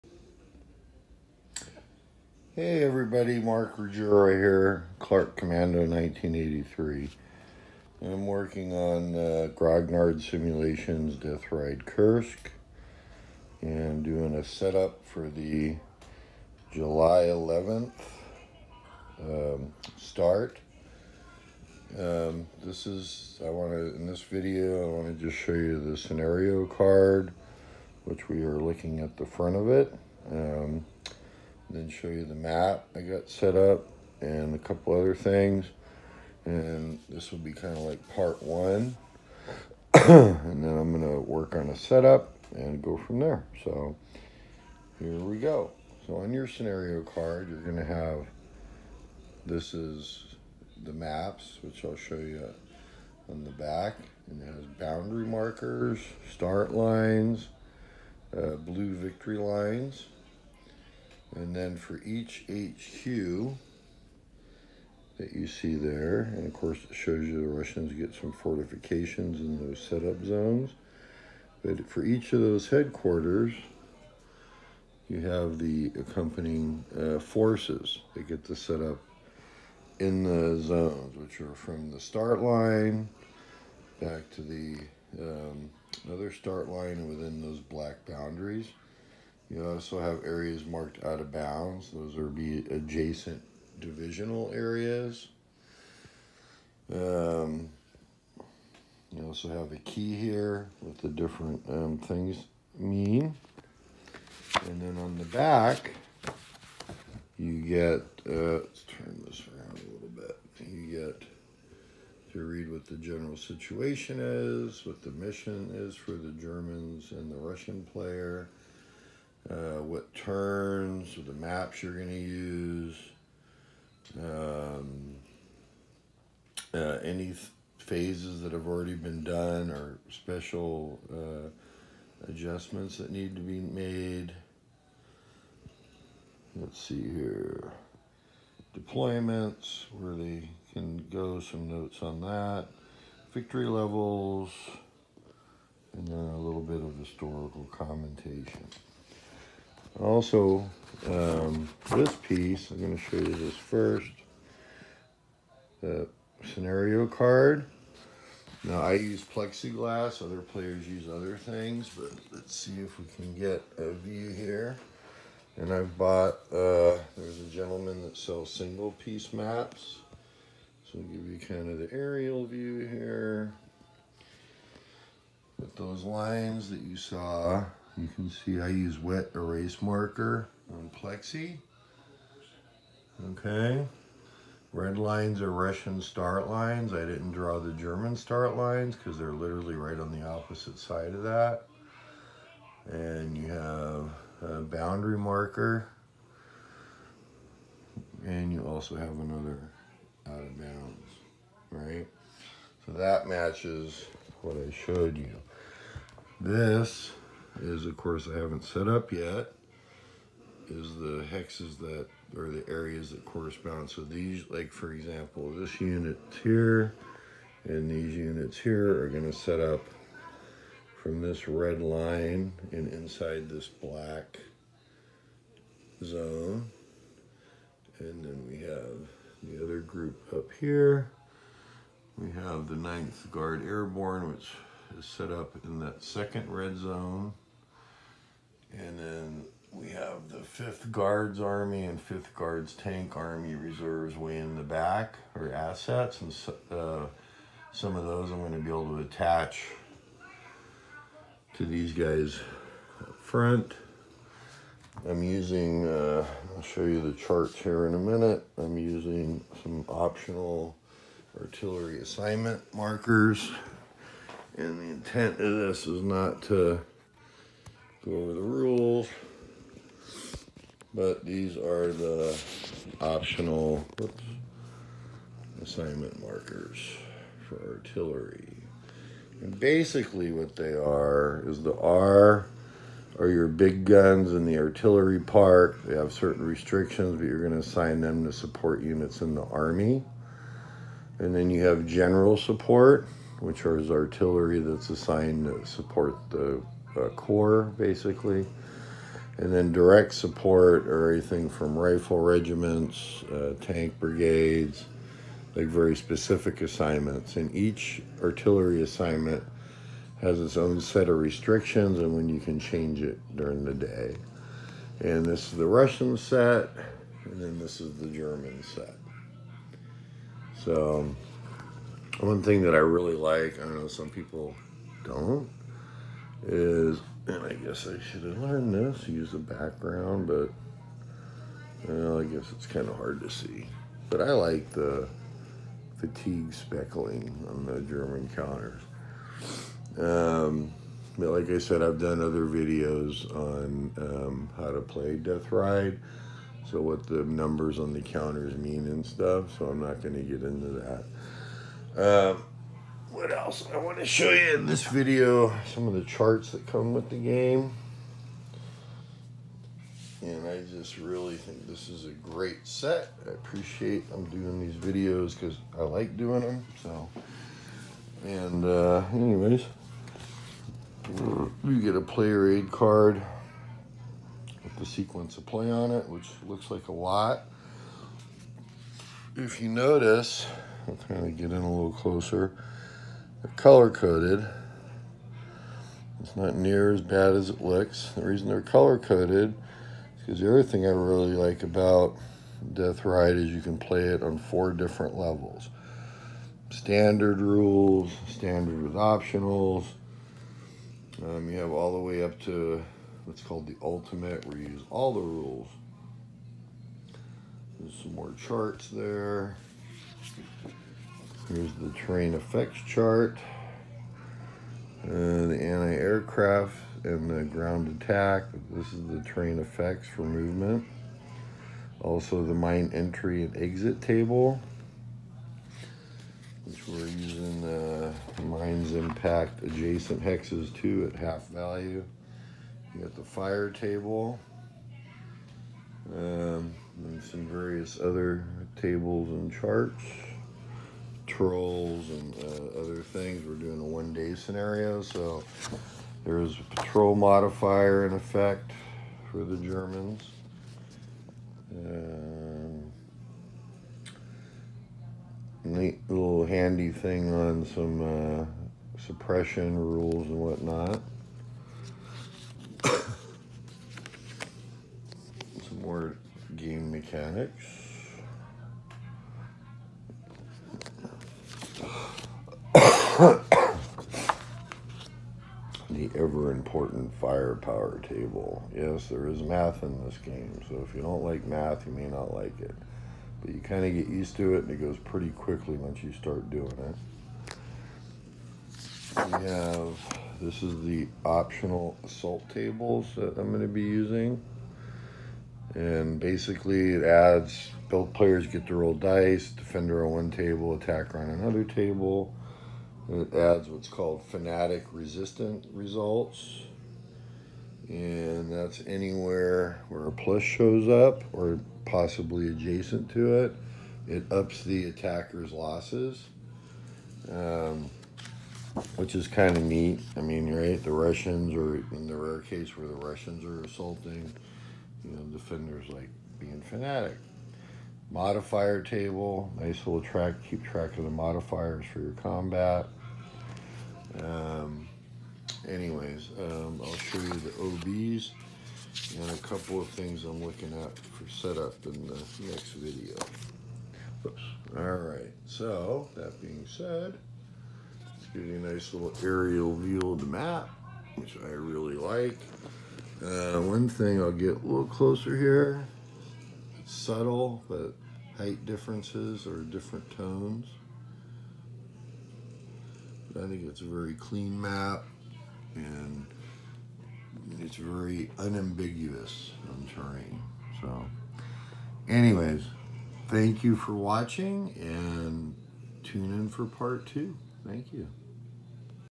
Deathride Kursk Leibstndarte 11 July part one
Setting up a scenario for play . Follow on will show set ups done a d possibly some play . This is video one.